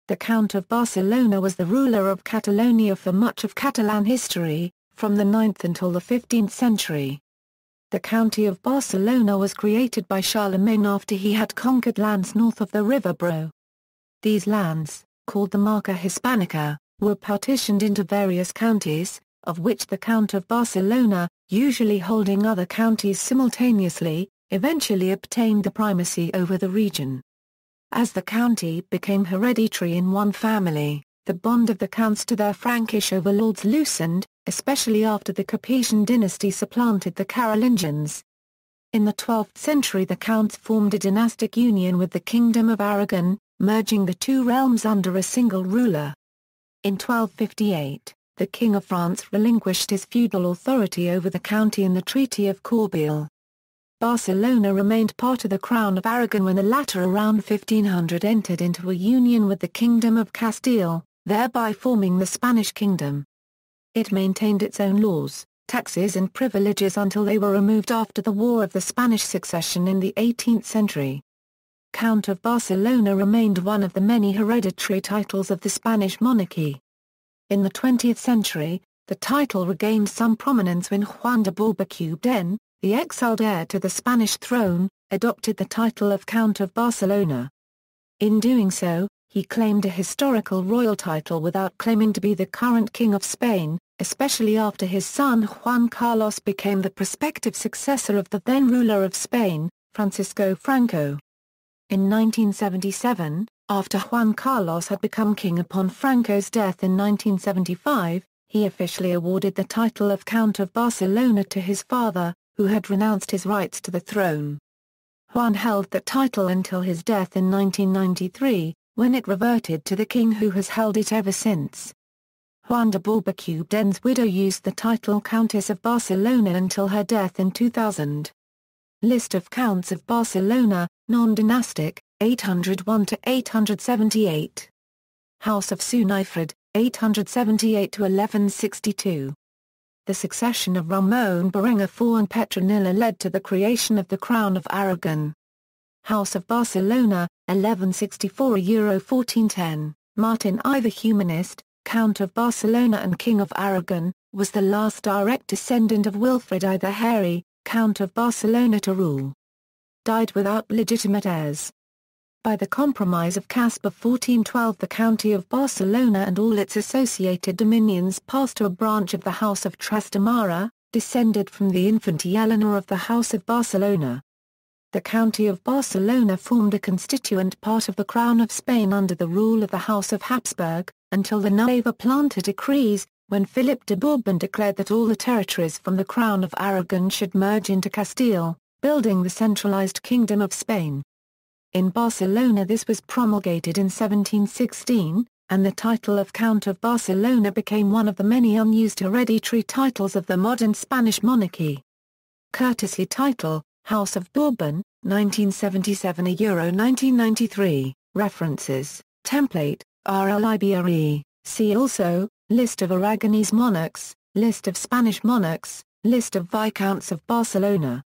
Count of Barcelona
The Count of Barcelona (Catalan: Comte de Barcelona, Spanish: Conde de Barcelona) was the ruler of Catalonia for much of Catalan history, from the 9th until the 15th century.
The County of Barcelona was created by Charlemagne after he had conquered lands north of the river Ebro. These lands, called the Marca Hispanica, were partitioned into various counties, of which the Count of Barcelona, usually holding other counties simultaneously, eventually obtained the primacy over the region.